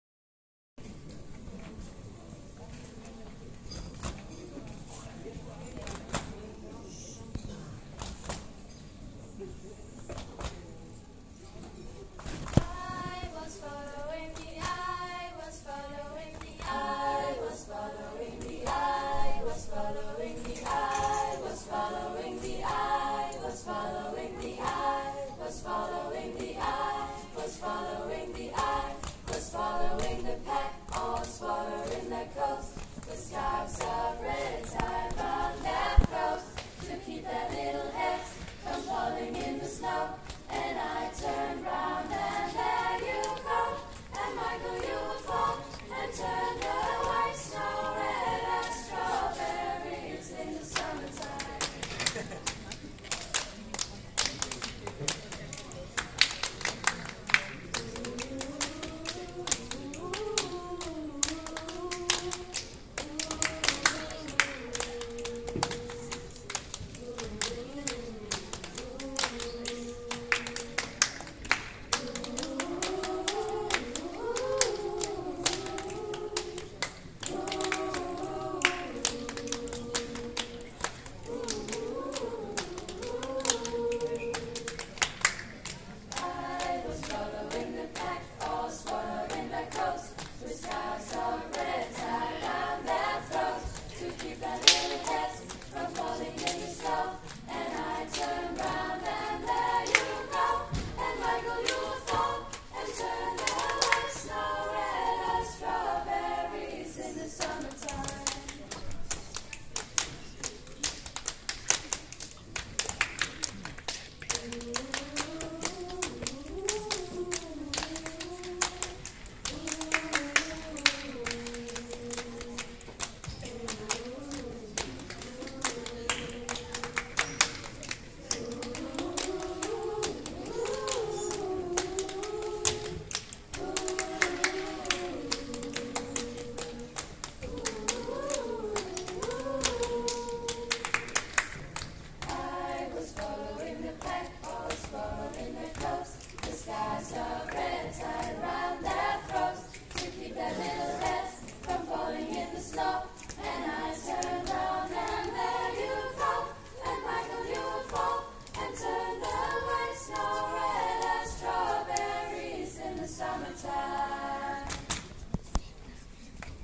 Hannah og DE koret laver bodypercussion
Min totalt seje datter laver bodypercussion på eget initiativ til adventshygge for alle forældre på Djurslands Efterskole. De lærte det på en uge.